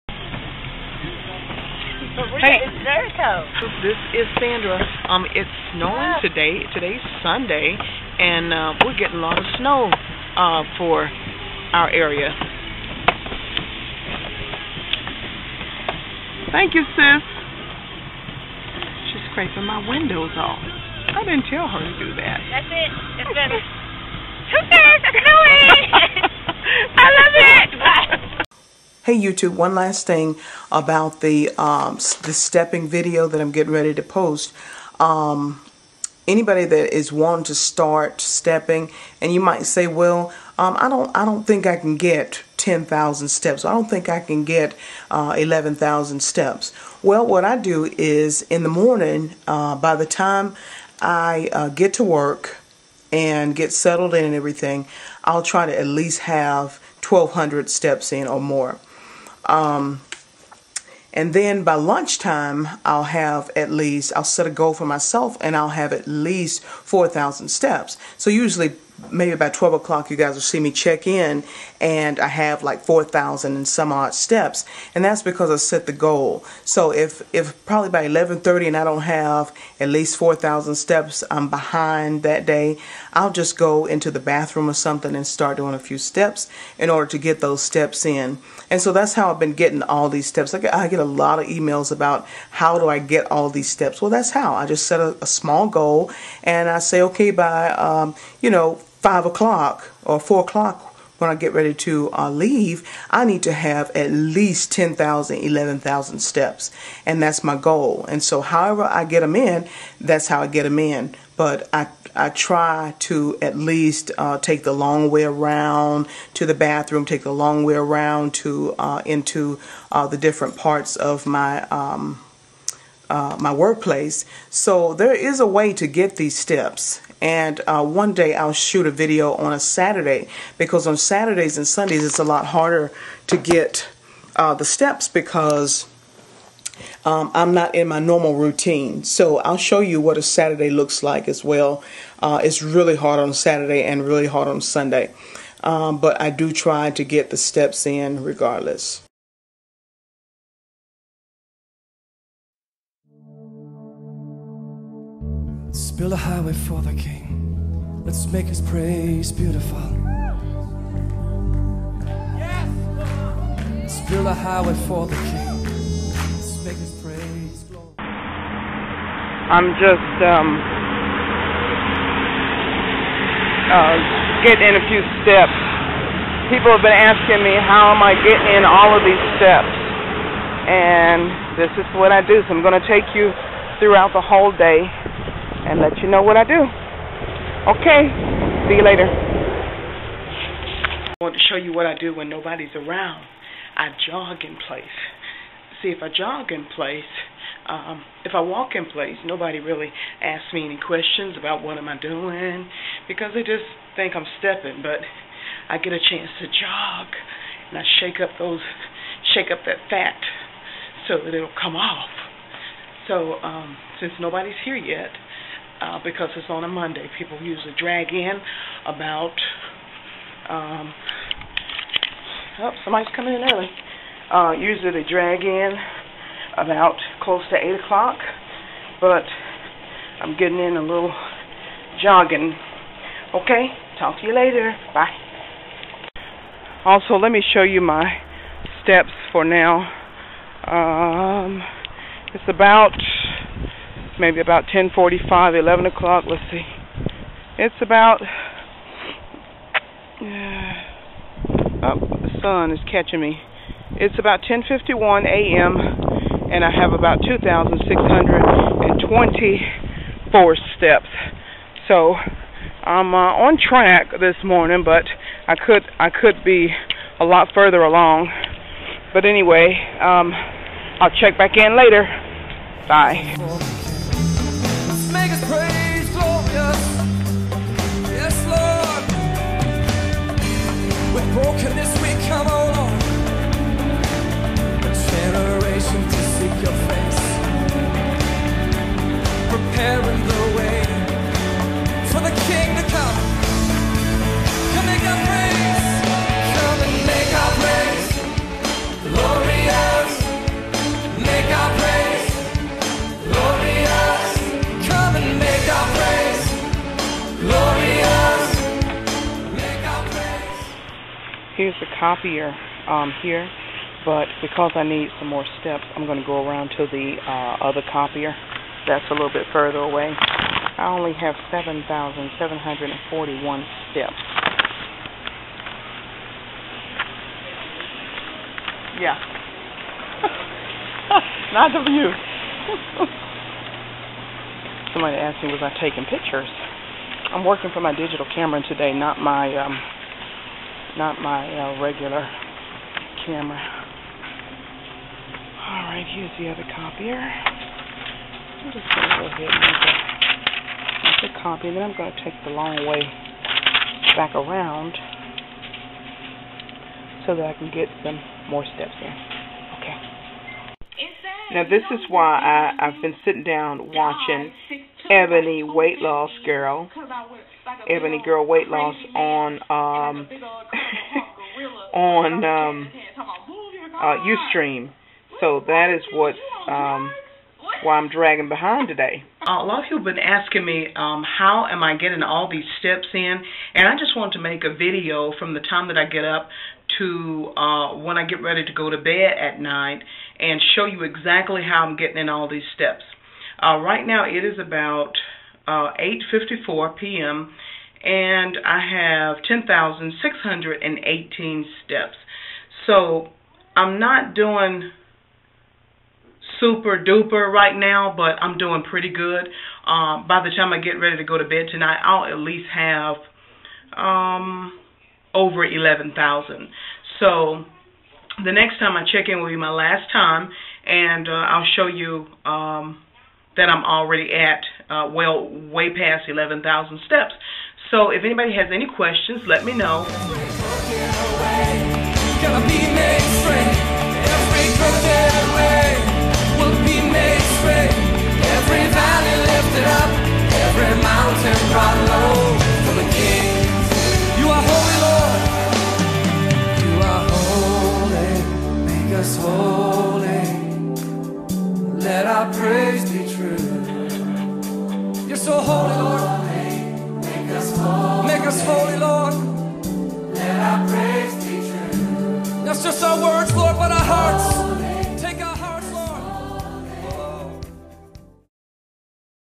How do you get 11,000 steps in a DAY!
I have received a lot of emails on stepping. I decided to go ahead a make a video to show that it is possible to get them in in 1 DAY :-)